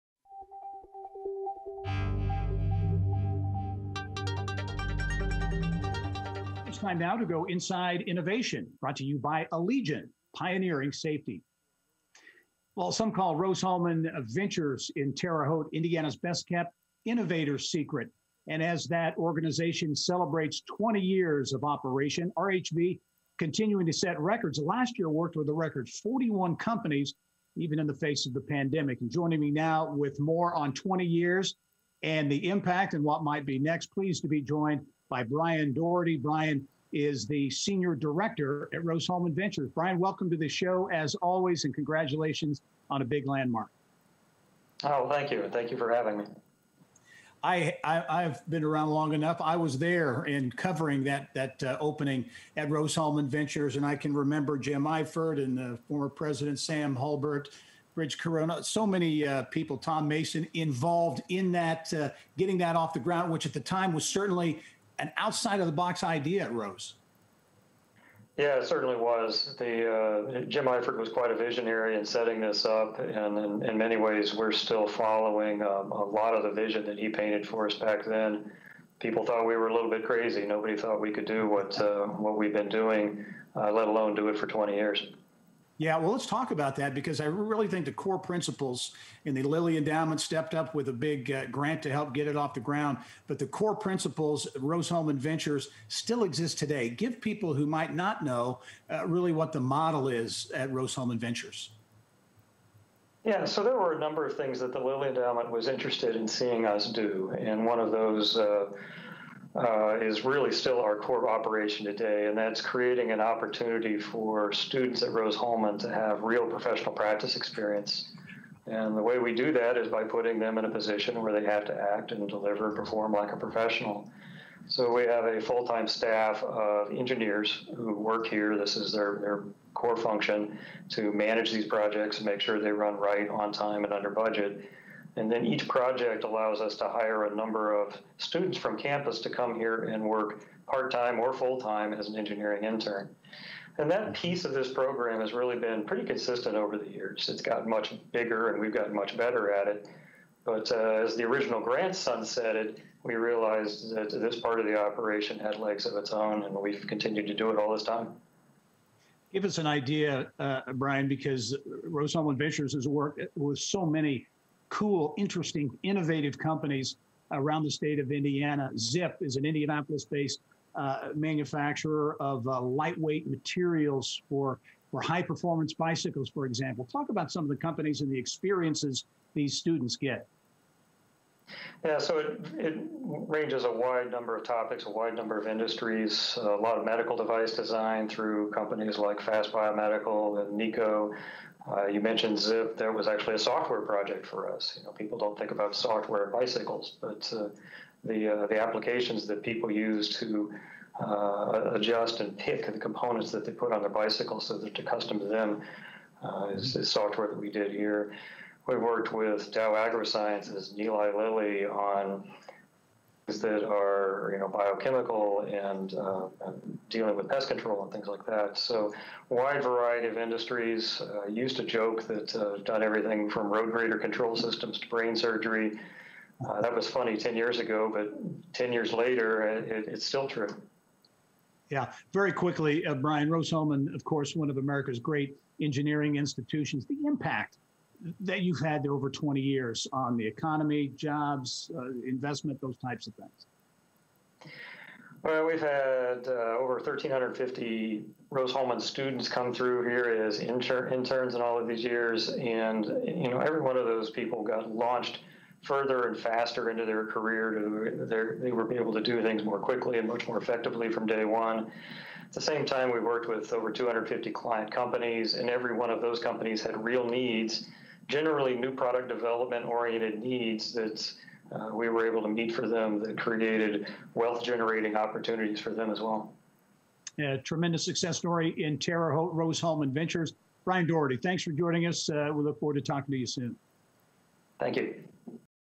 Rose-Hulman Ventures Celebrates 20 Years
Despite the pandemic, Rose-Hulman Institute of Technology’s Rose Hulman Ventures has experienced massive success in helping entrepreneurs turn their ideas into game-changing technology. Started 20 years ago through a $30 million Lilly Endowment Inc. grant, Rose-Hulman Ventures assisted a record 41 clients in 2020 and has worked on 175 projects over the last three years. We get details from Rose-Hulman Ventures Senior Director Brian Dougherty.